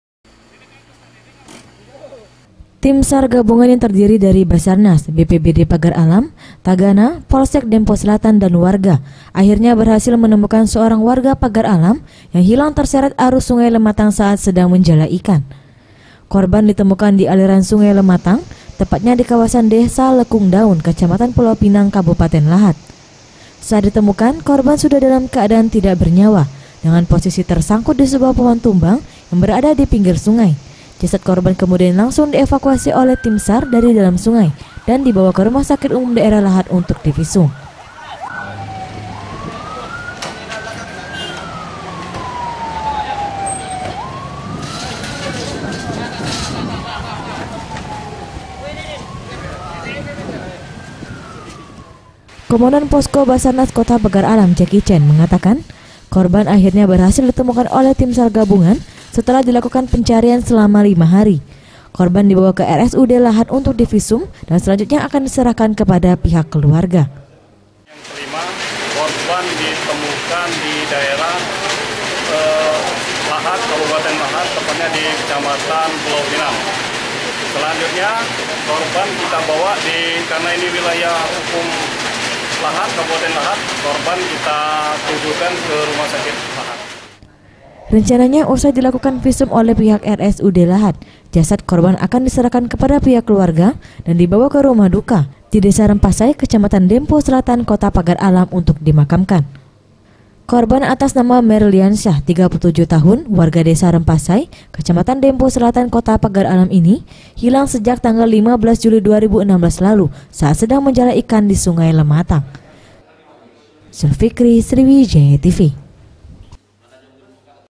Tim SAR Evakuasi Jasad Korban Hanyut Di Sungai Lematang
Setelah dilakukan pencarian selama lima hari, seorang warga Kota Pagaralam yang hilang terseret arus Sungai Lematang, akhirnya berhasil ditemukan oleh Tim SAR Gabungan. Saat ditemukan, kondisi korban sudah meninggal dunia, jasad korban pun kemudian langsung dibawa ke Rumah Sakit Umum Daerah Lahat untuk divisum.